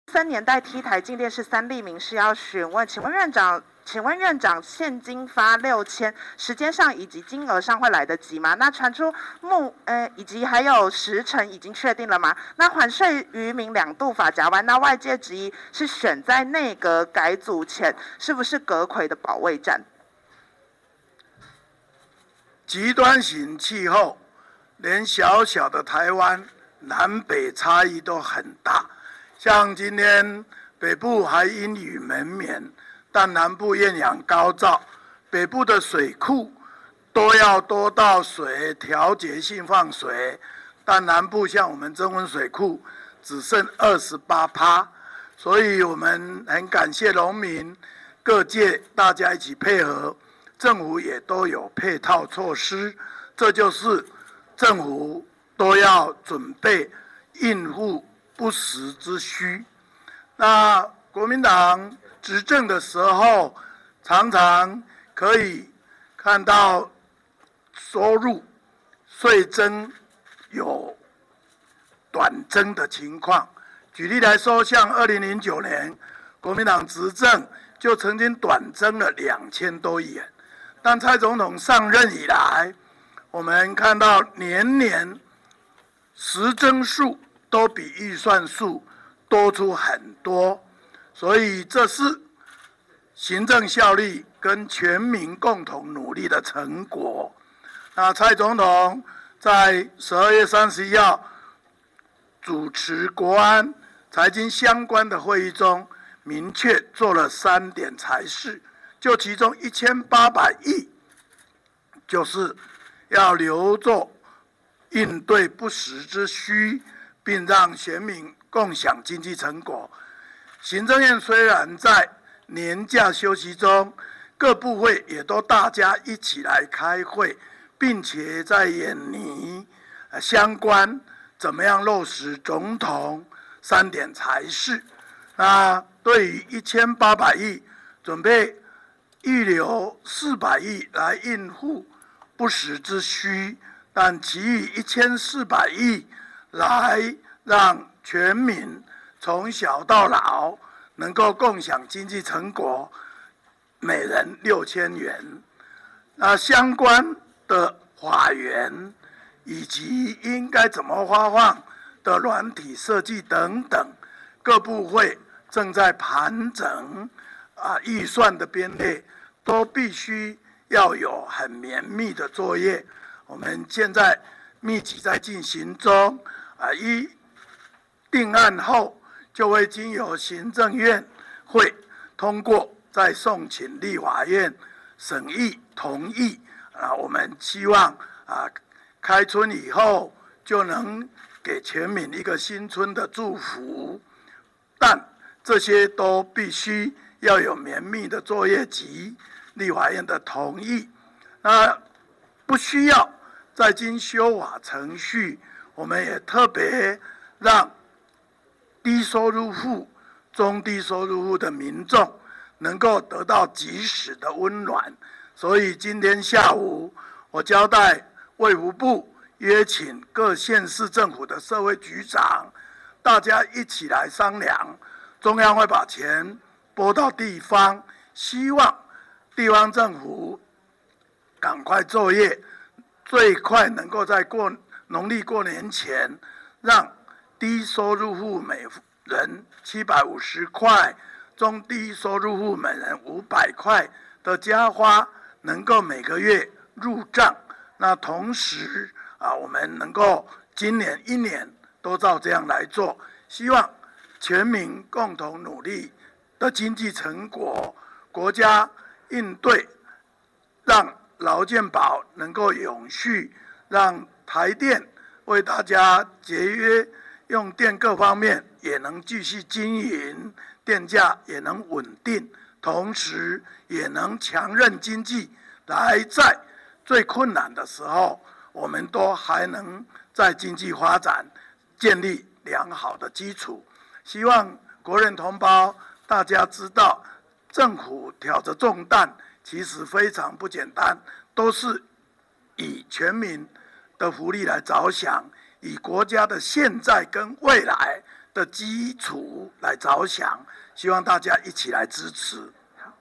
開春大紅包每人發6000元！蘇揆宣布全民共享經濟成果【最新快訊】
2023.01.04【最新快訊】總統蔡英文宣布去年全台稅收超徵，將透過「發現金」與全民共享。行政院蘇貞昌今（4）日上午在嘉義受訪證實，為了共享經濟成果，從小到老將發6000元，各部會正在盤整預算編列。